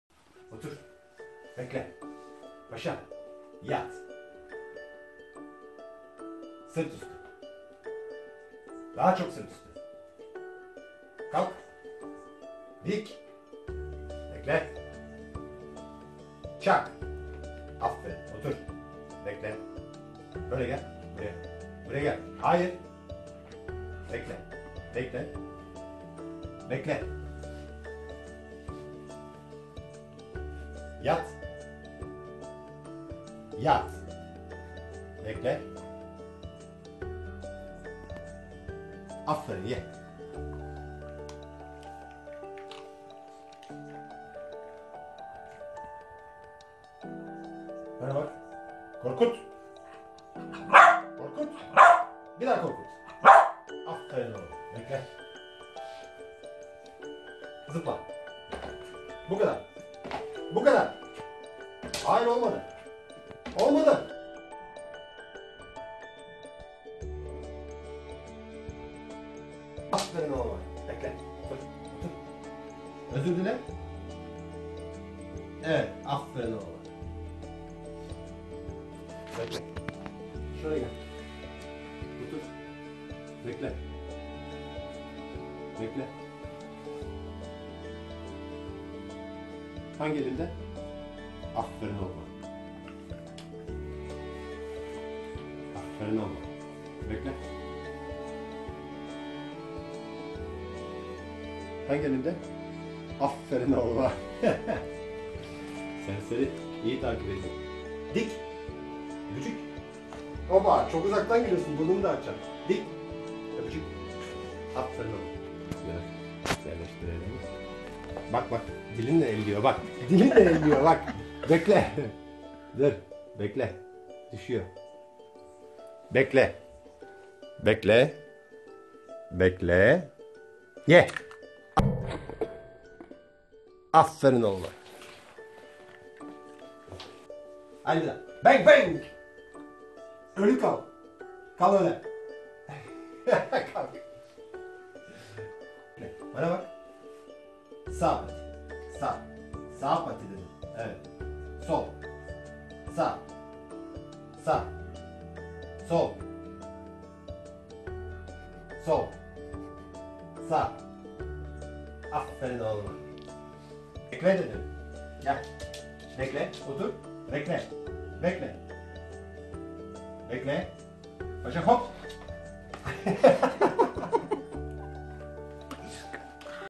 Yaşasın eğitim var, mama var, aferin var...
Maltese Terrier Paşa'nın eğitim videosu